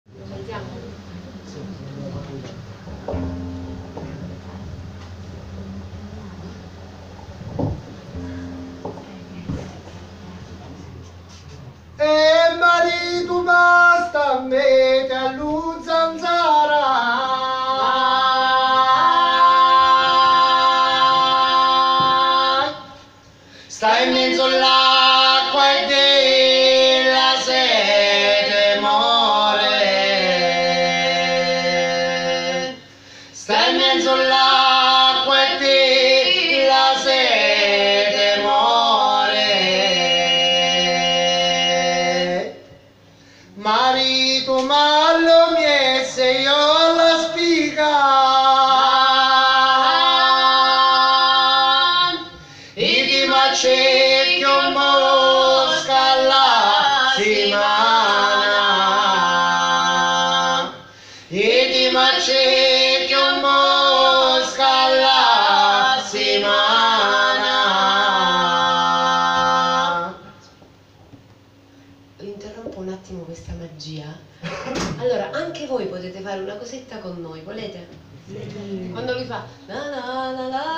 Enza Pagliara e Dario Muci - canto alla spiga
Roma, Club 55 Pigneto, 23 febbraio 2017, Enza Pagliara e Dario Muci eseguono un canto alla spiga raccolto da Dario presso le sorelle Gaballo di Nardò.